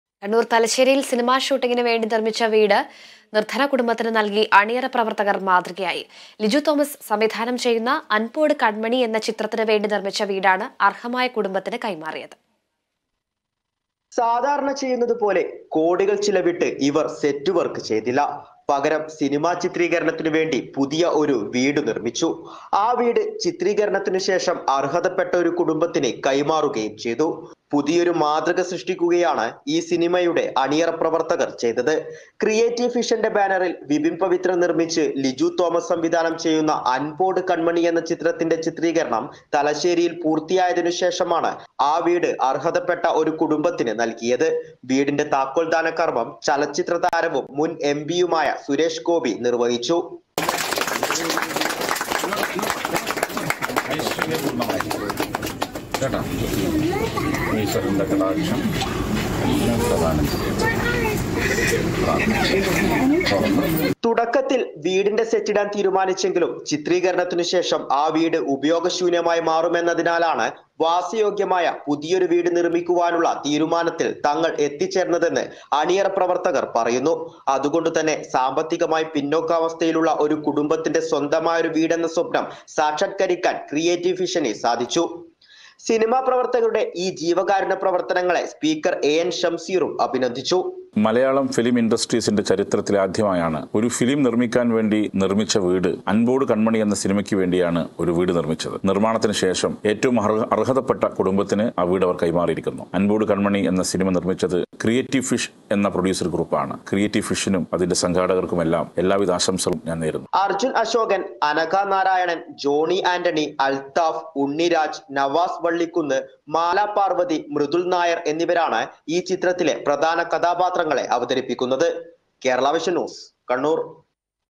സിനിമ ഷൂട്ടിങ്ങിനു വേണ്ടി നിര്‍മിച്ച വീട് നിര്‍ദ്ധനകുടുംബത്തിന് കൈമാറി | Kannur
കണ്ണൂര്‍ തലശ്ശേരിയില്‍ സിനിമാ ഷൂട്ടിങ്ങിനു വേണ്ടി നിര്‍മിച്ച വീട്  നിര്‍ദ്ധന കുടുംബത്തിന് നല്‍കി അണിയറ പ്രവര്‍ത്തകര്‍ മാതൃകയായി.
ലിജു തോമസ് സംവിധാനം ചെയ്യുന്ന 'അന്‍പോട് കണ്‍മണി 'എന്ന ചിത്രത്തിന് വേണ്ടി നിര്‍മിച്ച വീടാണ് അര്‍ഹമായ കുടുംബത്തിന് കൈമാറിയത്

About COA